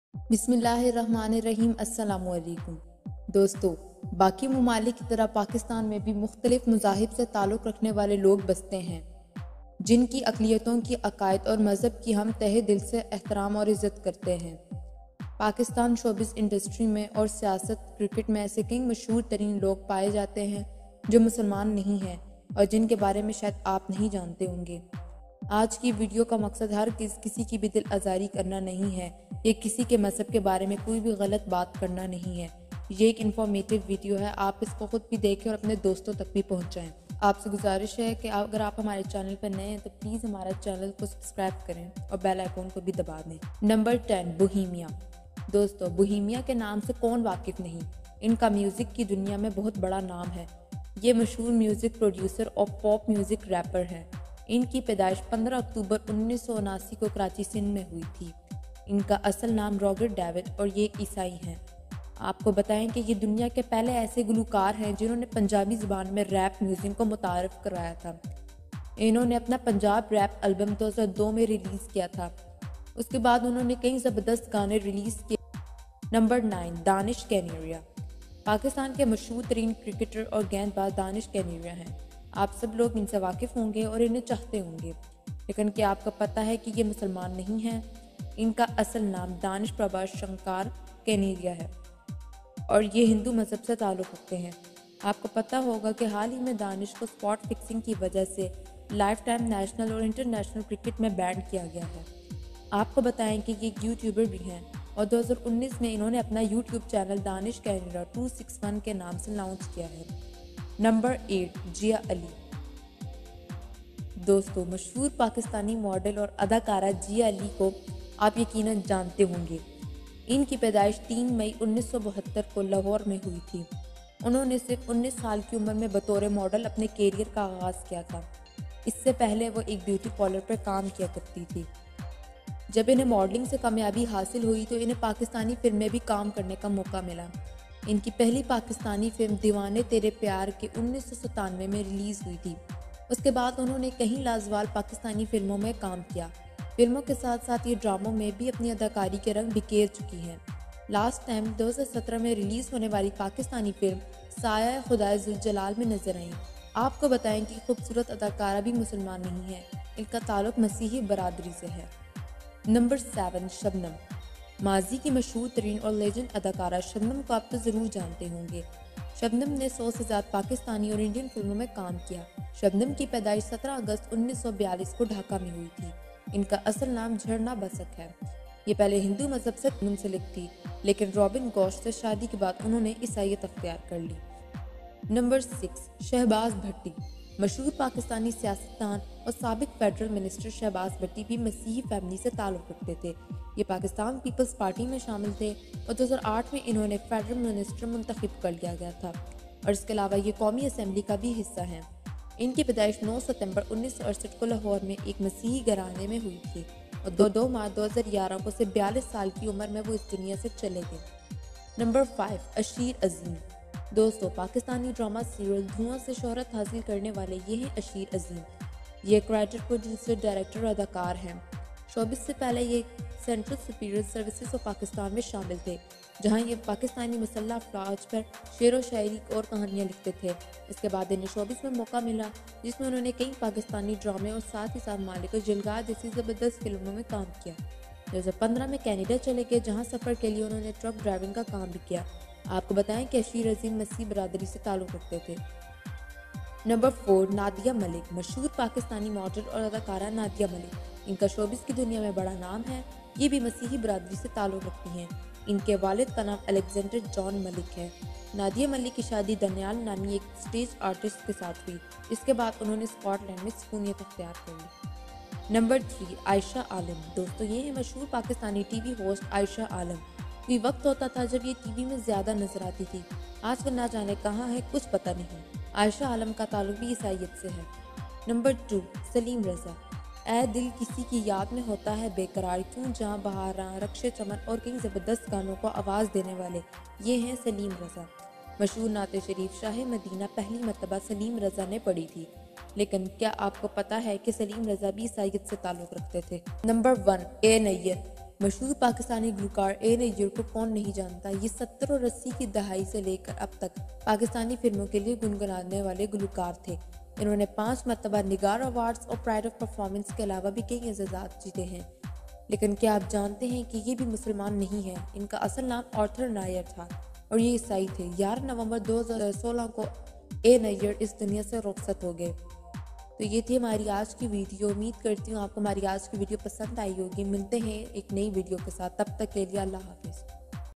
Top 10 pakistani celeberities who are not muslims | مشہور پاکستانی سٹار جو مسلمان نہیں ہیں۔
Welcome to Amir info tv